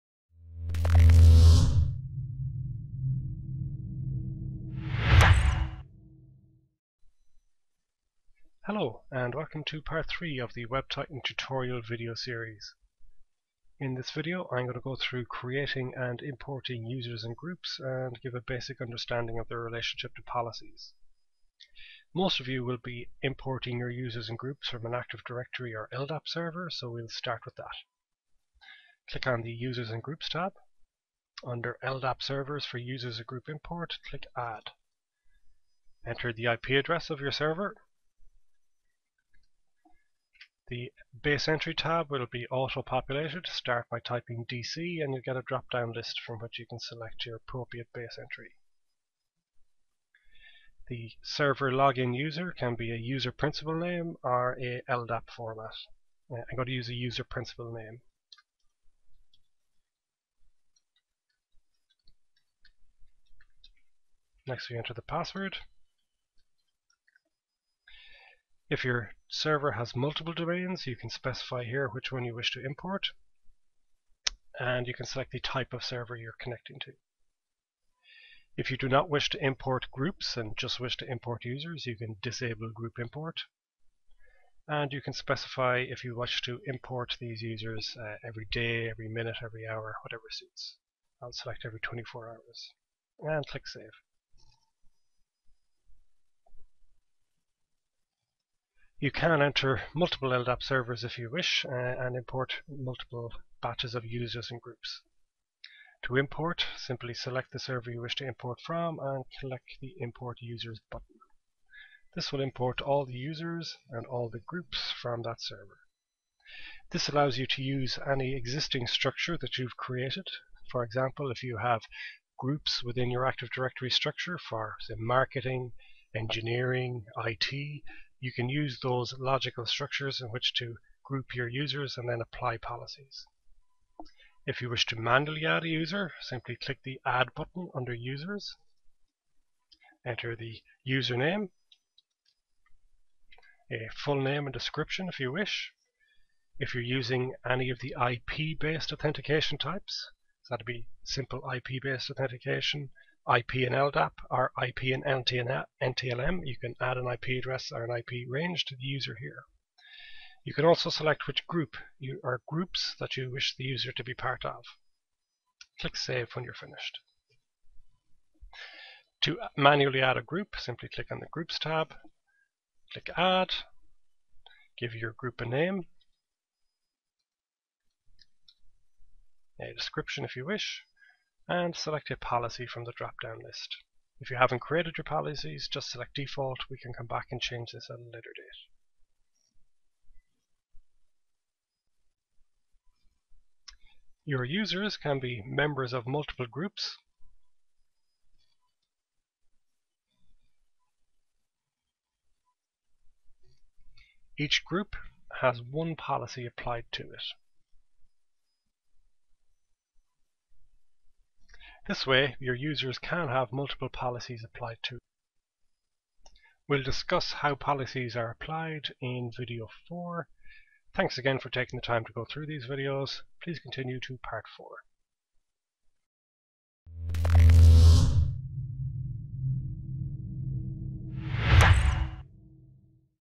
WebTitan 5.00 Set Up: Part 3 - Users groups
In this video we will go through importing users and groups, and give a basic understanding of their relationship to policies.